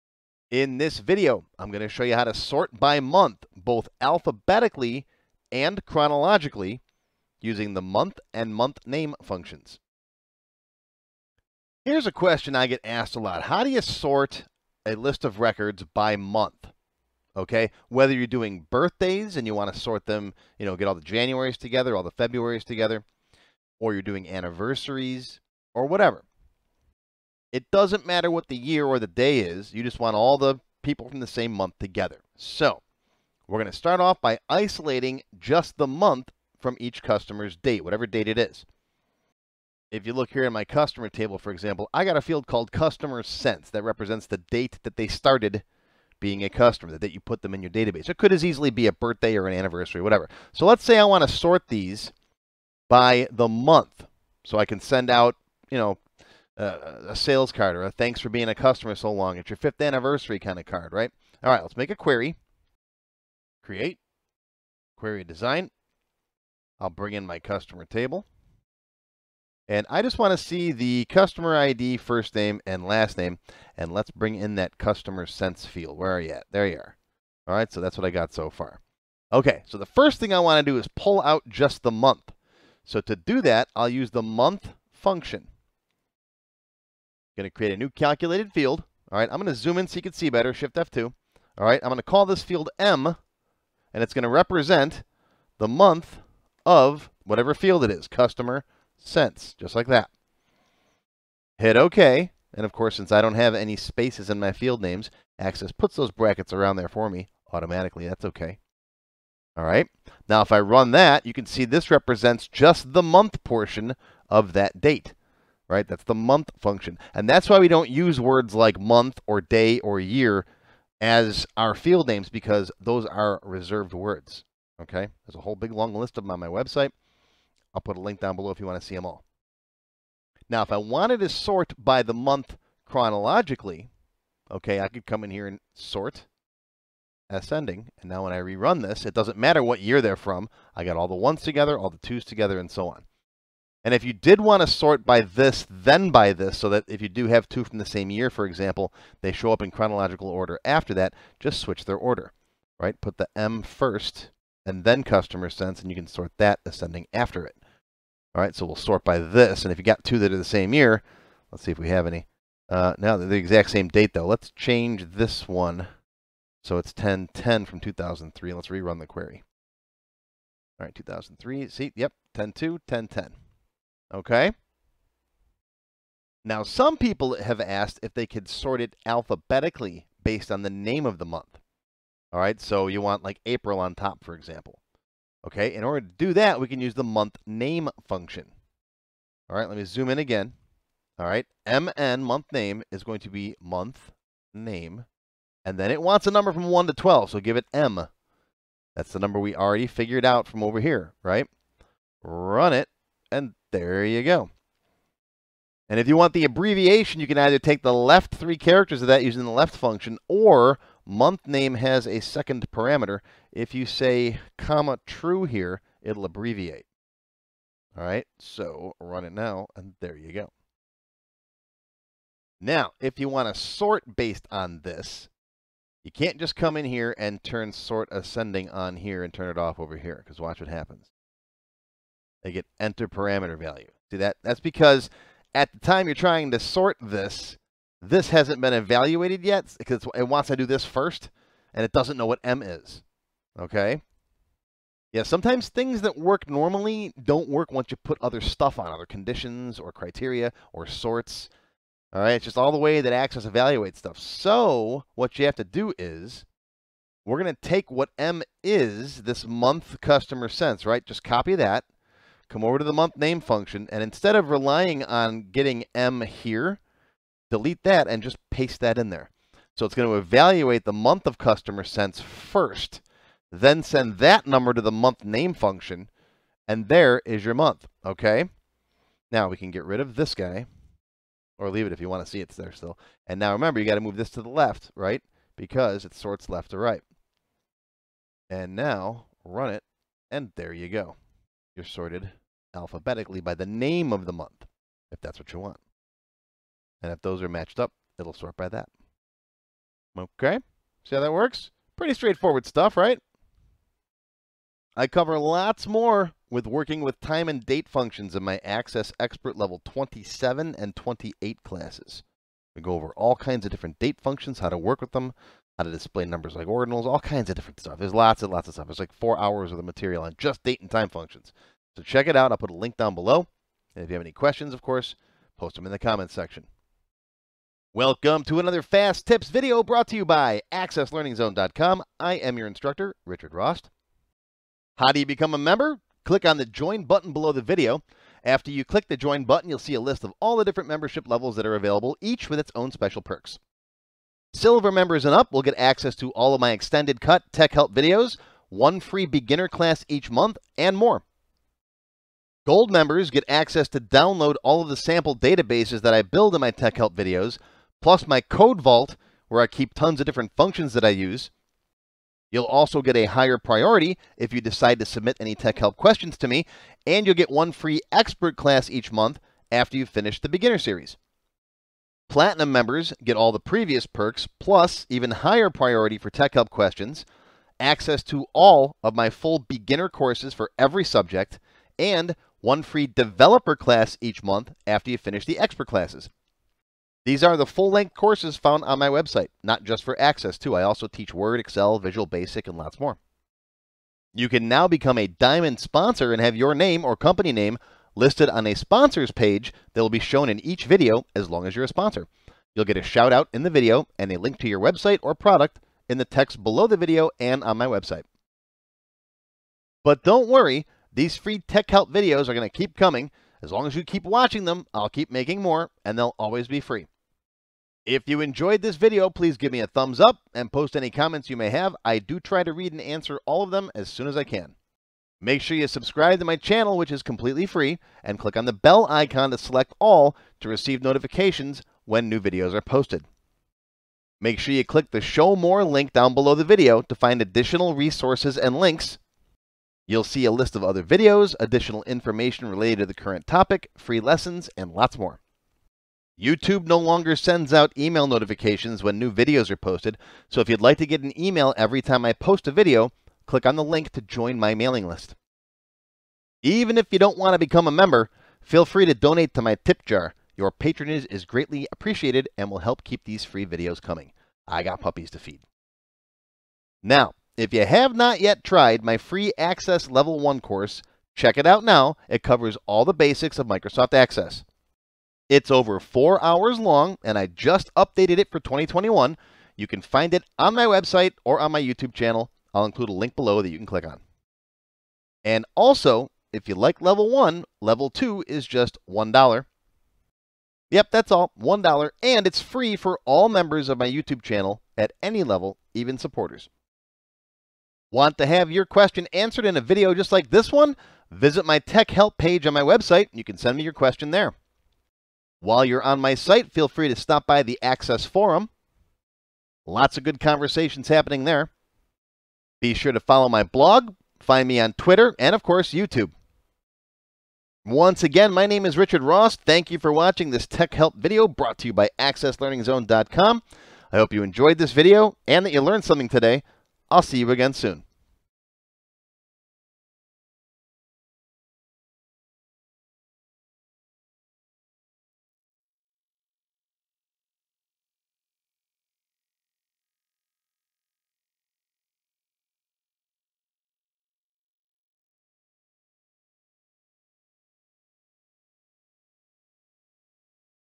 How to Sort by Month in Microsoft Access (Alphabetically and Chronologically) Using Month, MonthName
In this Microsoft Access tutorial, I'm going to show you how to sort by the month of any date, both alphabetically and chronologically, using the Month and MonthName functions.